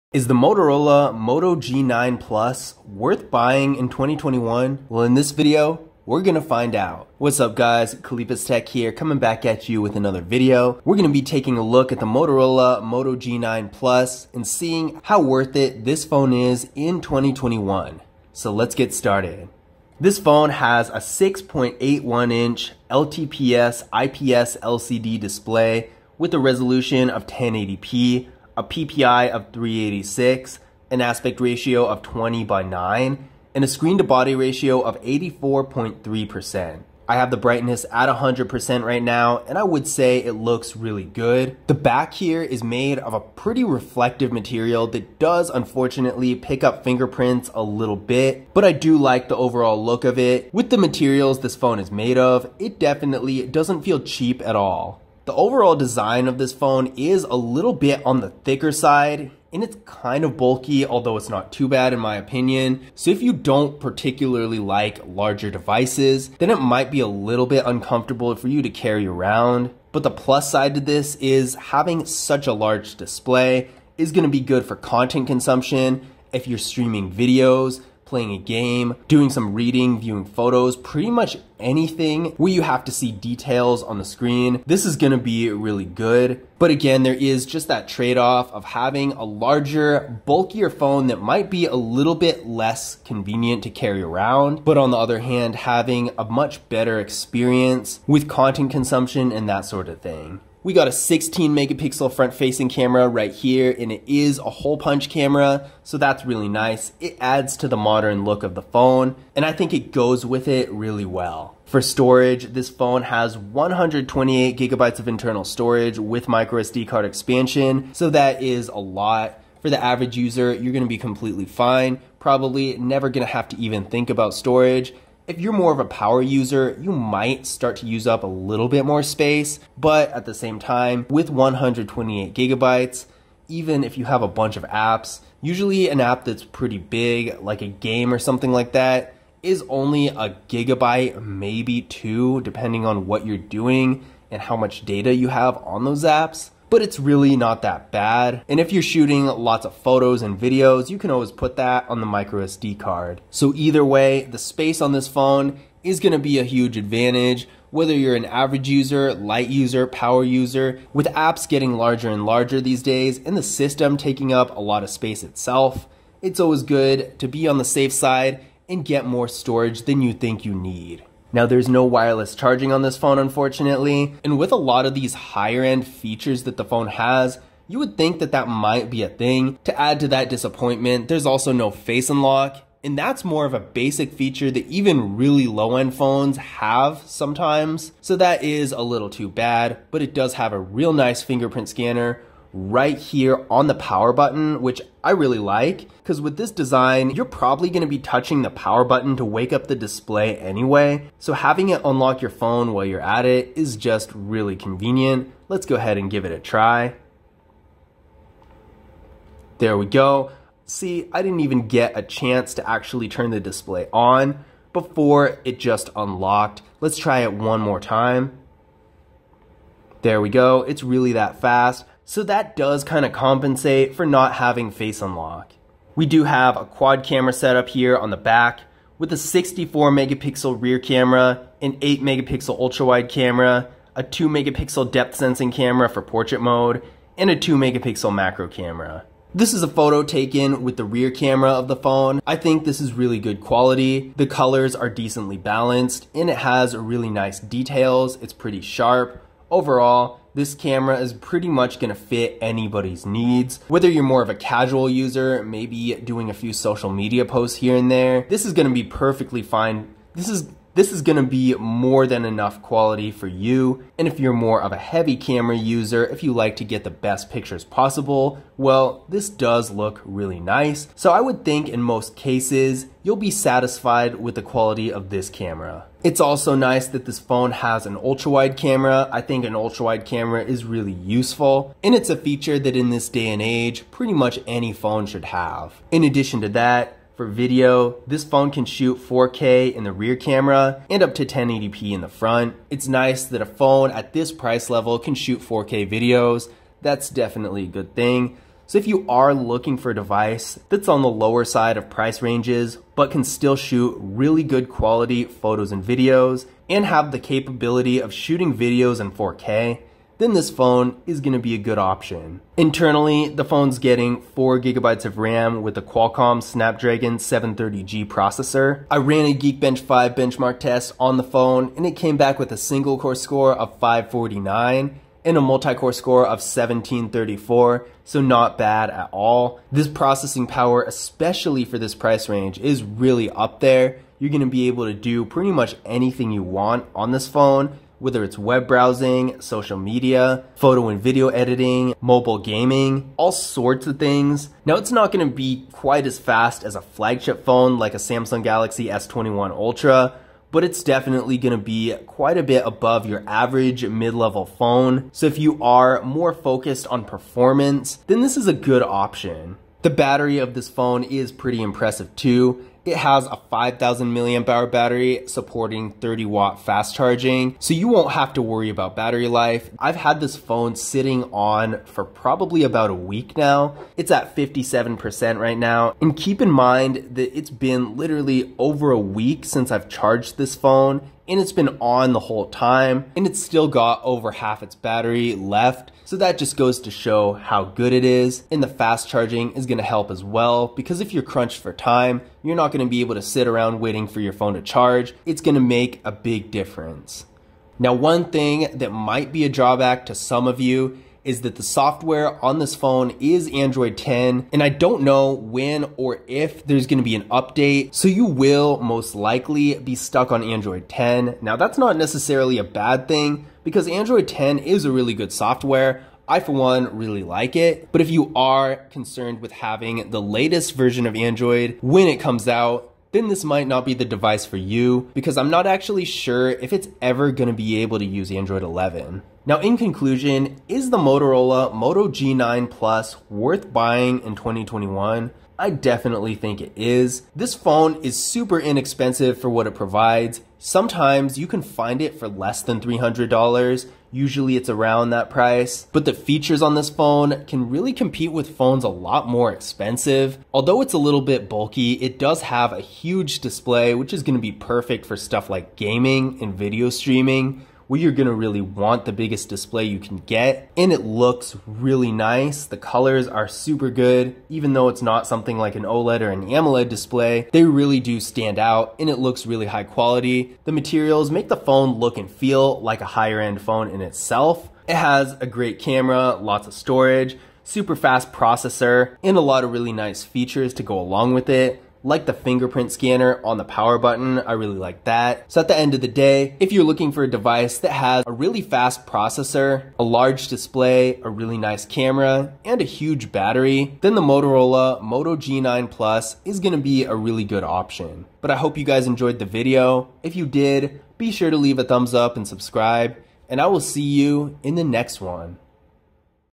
Motorola Moto G9 Plus - Is It Worth It In 2021?
Here we go over whether or not the Motorola Moto G9 Plus is worth buying in 2021.